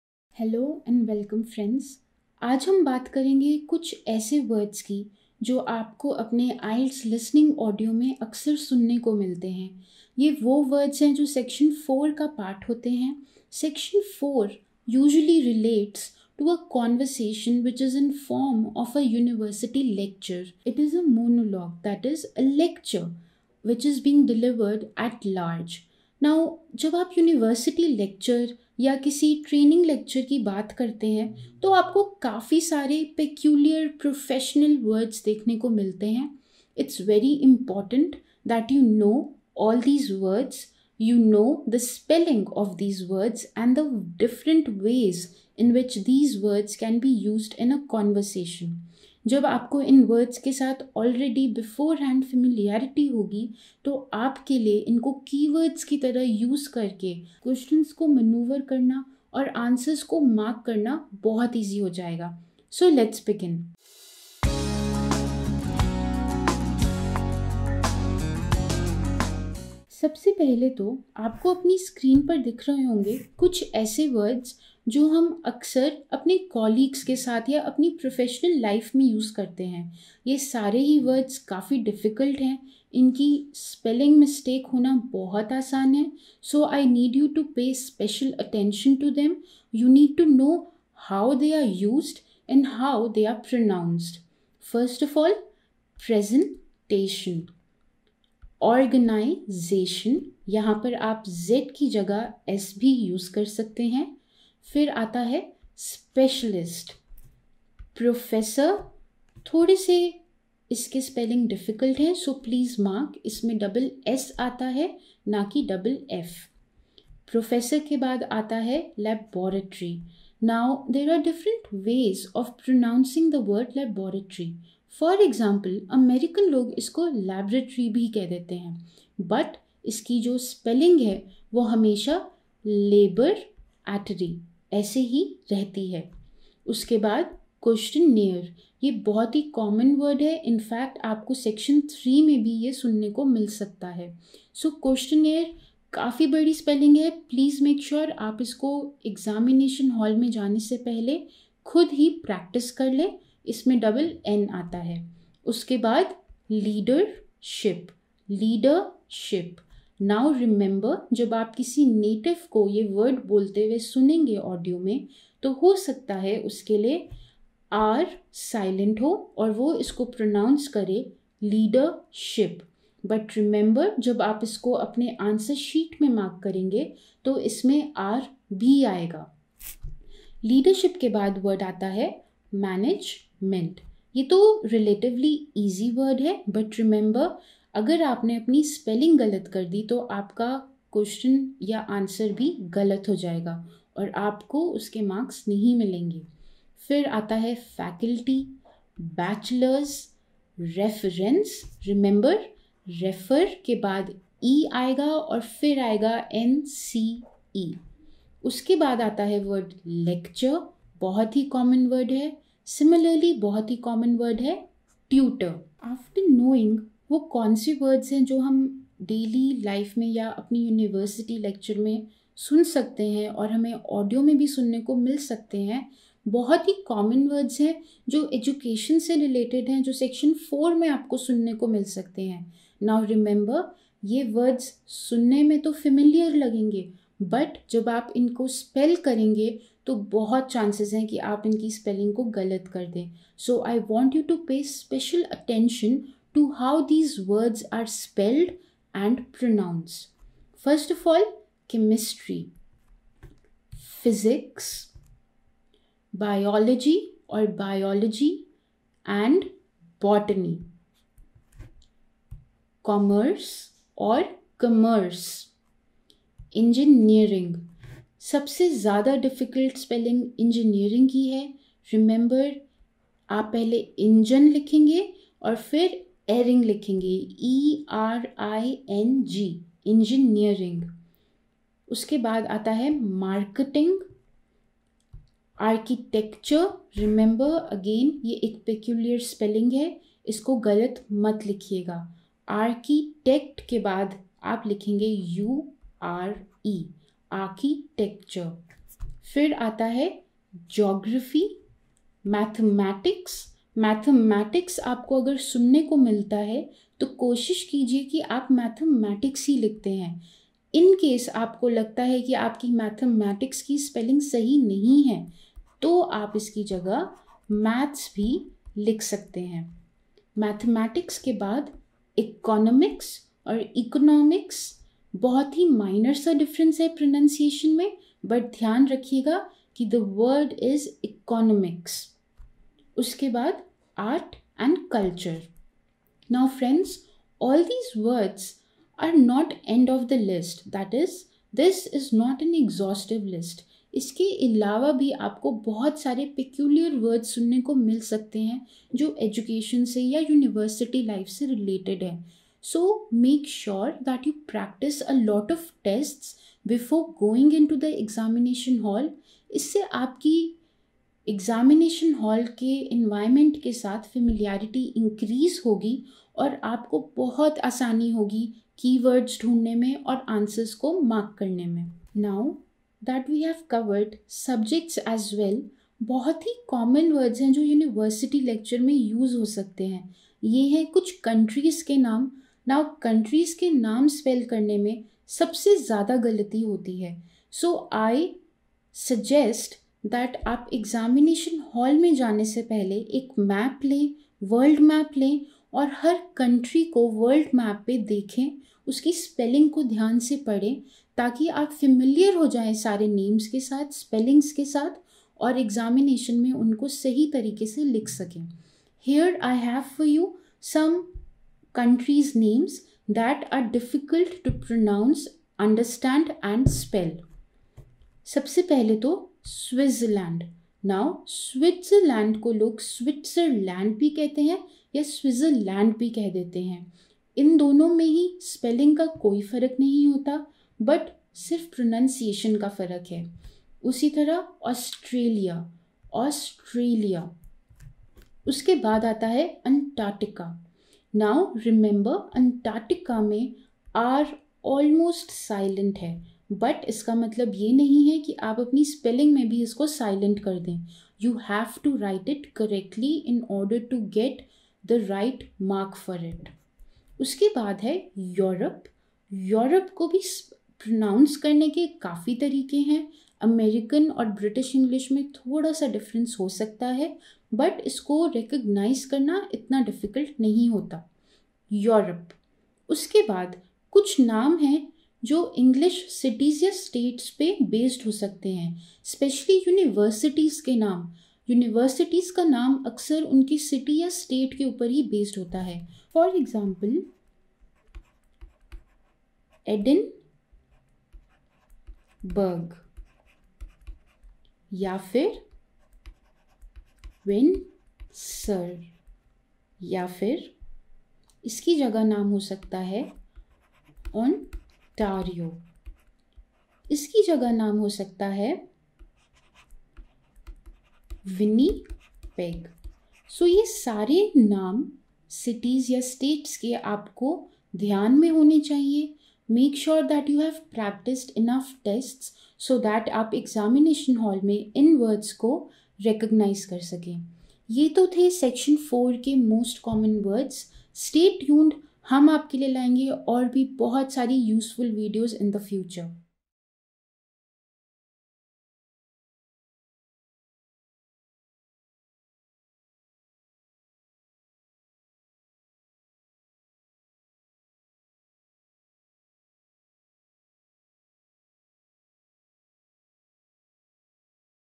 IELTS Listening | IELTS Course For Preparation | Section 4 Listening | Tricks For IELTS Exam
This English video for IELTS Exam Preparation will let you know How to Score Well in your Listening exam? by revealing Section 4 Listening that will help you excel in your examination